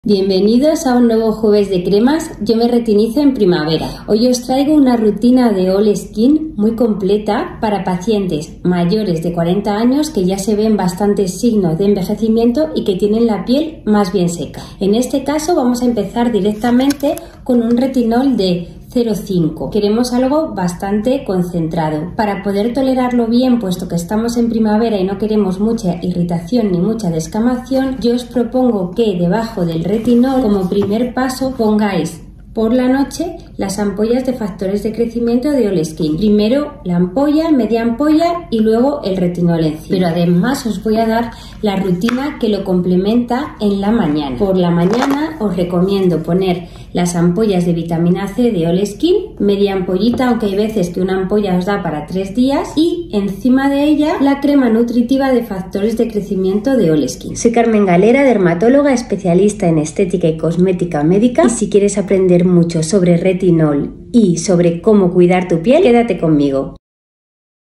Retinol para mayores de 40 años con piel seca | Dermaforyou by Dra Carmen Galera
Tienes signos de envejecimiento varios???
👉🏻manchas?
👉🏻arrugas?
👉🏻flacidez?

¿Quieres una rutina que realmente te pueda ofrecer resultados? 🤞🏻
¿No puedes permitirte un presupuesto muy muy alto? 💶💶
La rutina que te explico en el vídeo te cambiará la piel, cuéntamelo 🙏

Tlf de Cita Previa: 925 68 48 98⁣
WHATSAPP- SOLO MENSAJES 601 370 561⁣
📍Avda. Constitución, 8⁣
Talavera de la Reina⁣⁣⁣⁣